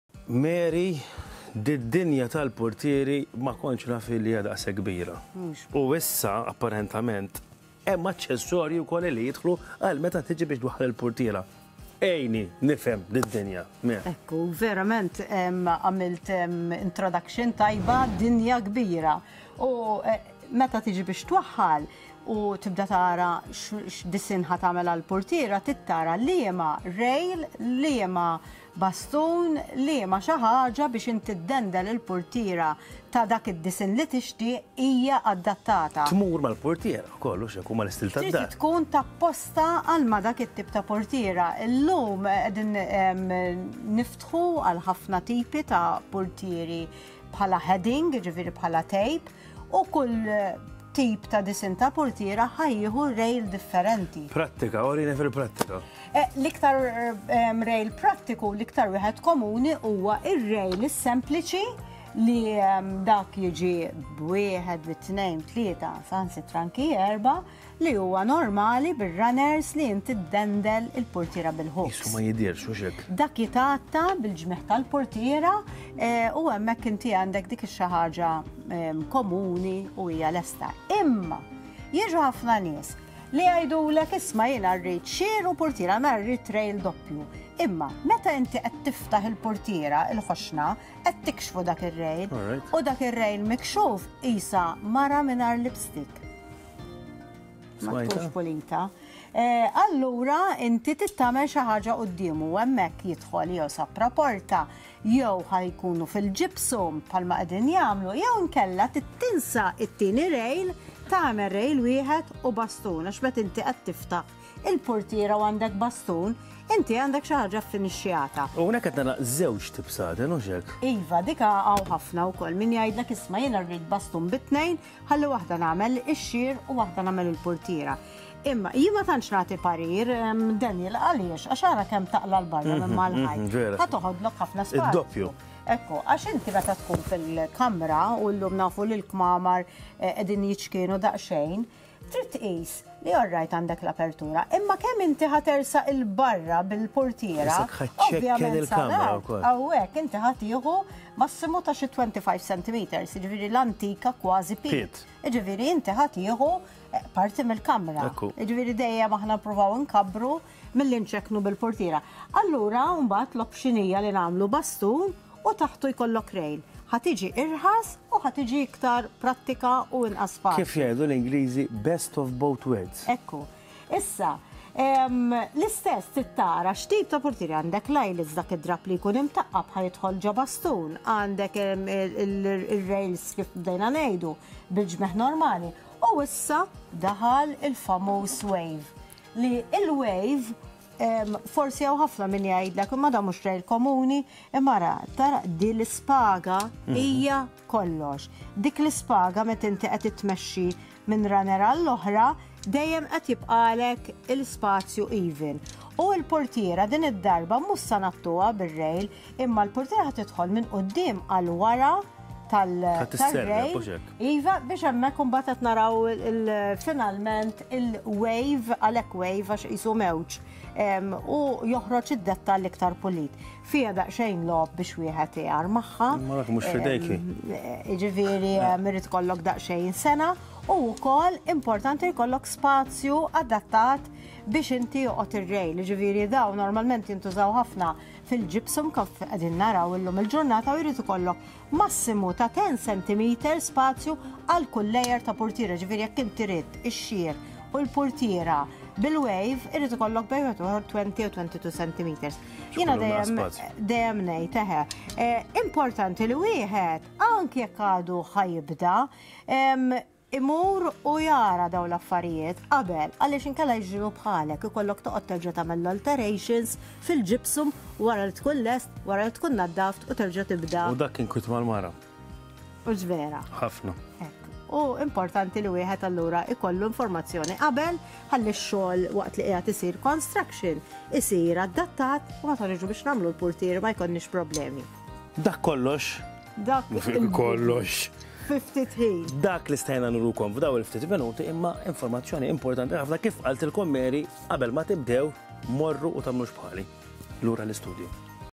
How to take care of the curtains and adhere specific accessories to them?
Mary Tabone from Home Interiors advises Maya on the importance of separating colours in the wash at a temperature of not more than 30 degrees. Mary shares more tips via this feature.